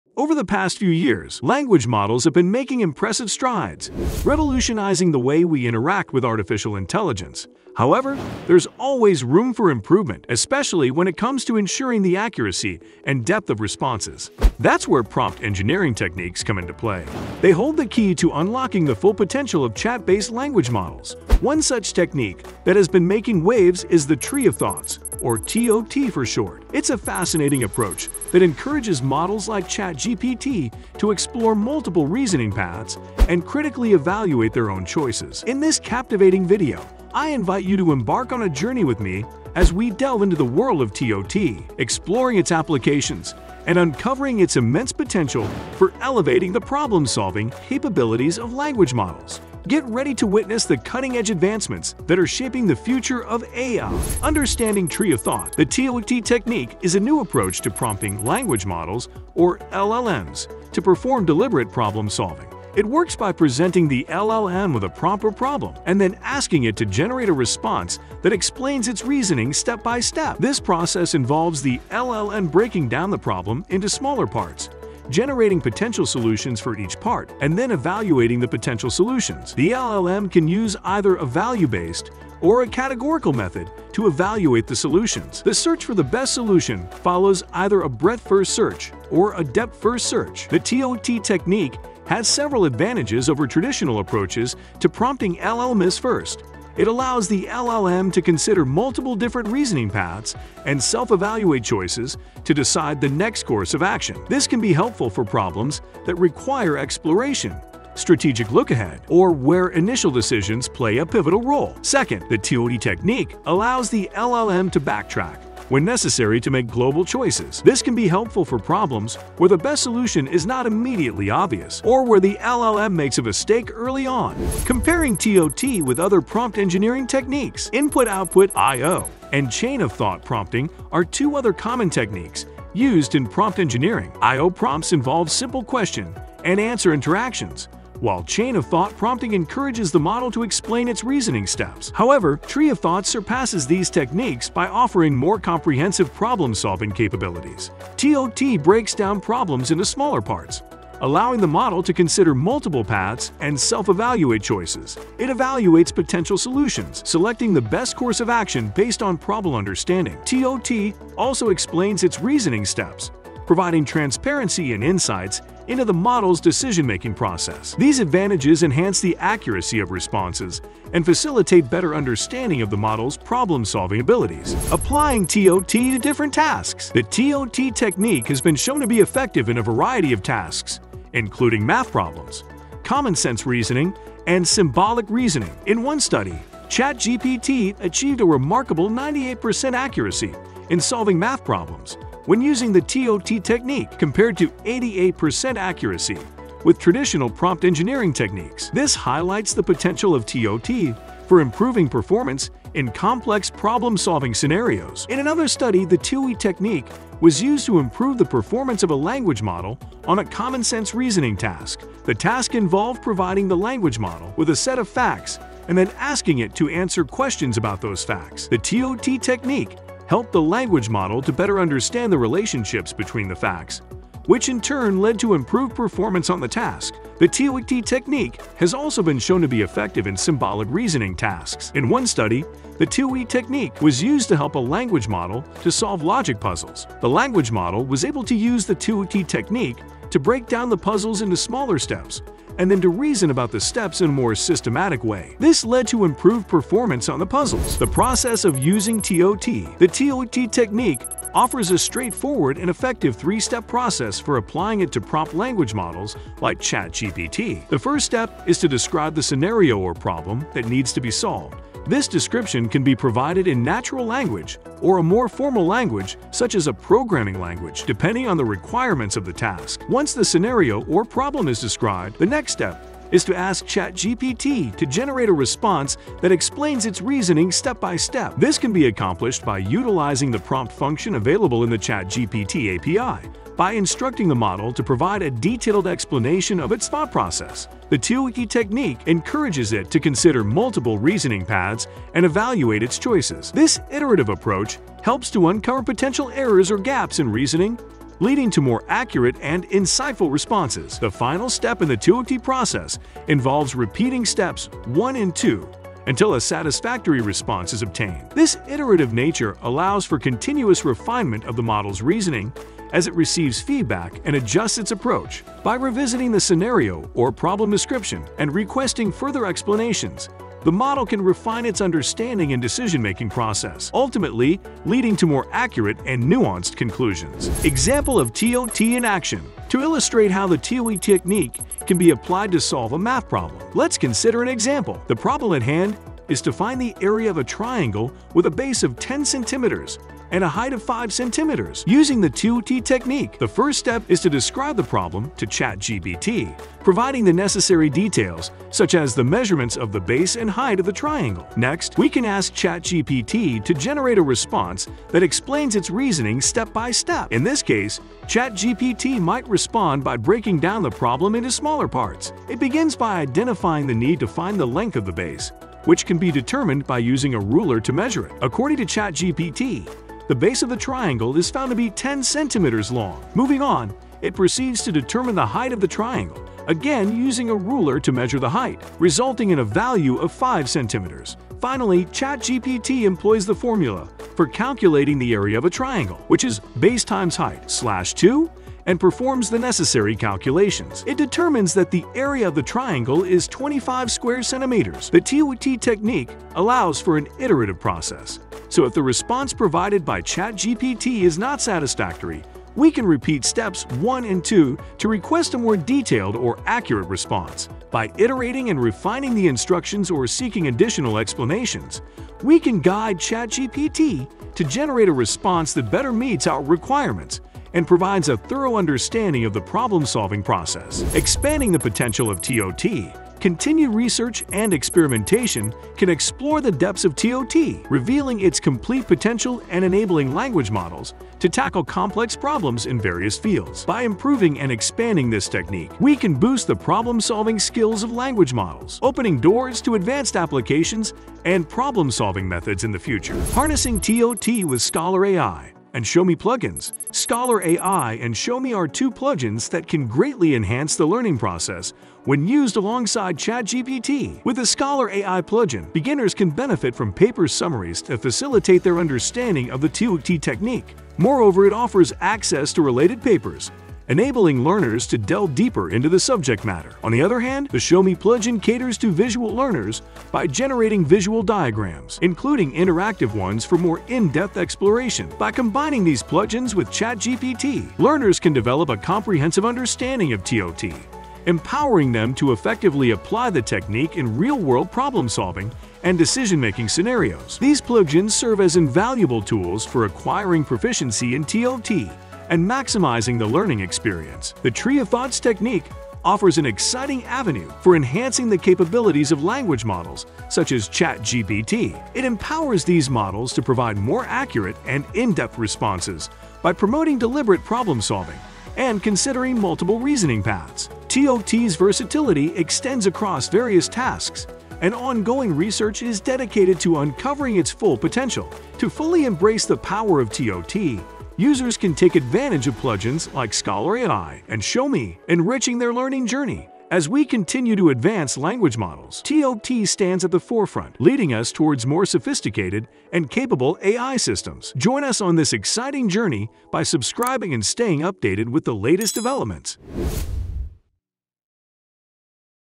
Unlocking Language Models: The Power of Prompt Engineering
Over the years, language models have redefined AI interactions, but there's always room for growth. Join us in this captivating video as we delve into the revolutionary Tree of Thoughts (TOT) technique, unlocking the true power of chat-based language models like ChatGPT. 🚀🤖

🌿 Understanding TOT: Discover how the Tree of Thoughts approach encourages models to explore multiple reasoning paths and self-evaluate choices, elevating their problem-solving capabilities. Learn how it breaks down problems into smaller parts, enabling comprehensive exploration.

💡 Comparing Techniques: Dive into a comparison of TOT with other prompt engineering techniques like IO and chain of thought prompting. See why TOT stands out by promoting deeper problem understanding and transparent decision-making.

🔬 Applying TOT to Tasks: Explore TOT's effectiveness in math problems, commonsense reasoning, and symbolic reasoning tasks. Witness how TOT helps models achieve remarkable accuracy and performance improvements.

📝 The TOT Process: Learn a simple three-step process to apply TOT effectively to prompt language models. Understand how iterative refinement leads to more accurate and insightful responses.

🎯 Example in Action: Follow an example of using TOT to solve a math problem, revealing how the model reasons step-by-step and refines its approach for better results.

🚀 Expanding TOT's Potential: Uncover the limitless possibilities of TOT as ongoing research pushes the boundaries of language model capabilities.

🔌 Harnessing Scholar AI and Show Me: Discover the power of Scholar AI and Show Me plugins, enriching your learning experience and proficiency in TOT.

The Tree of Thoughts technique is reshaping the future of AI problem-solving. Let's unlock the full potential of language models together! 🌳🧠💻